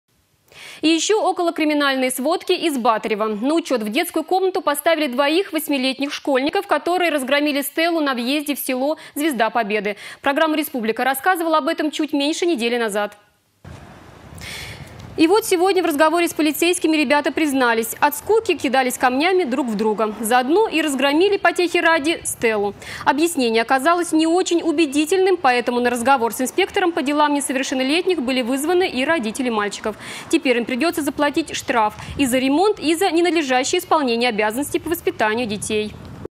Стелу в Батырево повредили школьники
На учет в детскую комнату поставили двоих восьмилетних школьников, которые разгромили стелу на въезде в село «Звезда победы» Программа «Республика рассказывала об этом  чуть меньше недели назад.